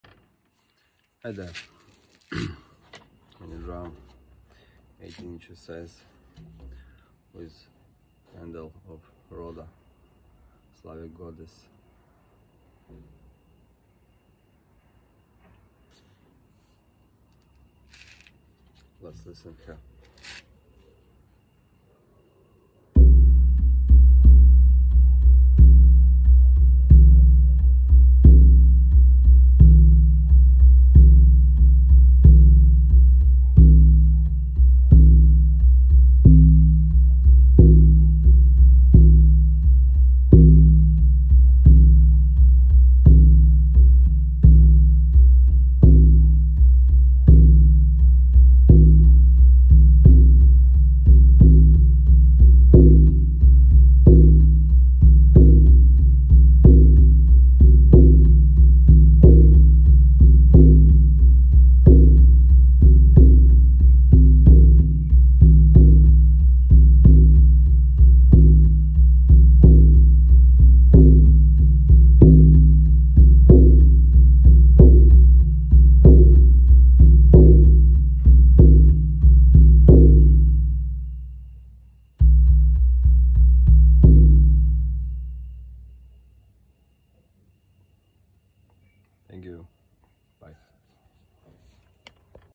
Shaman drum Roda goddess
18 inches
Goat skin
Ash wood rim
Beater included
For sale…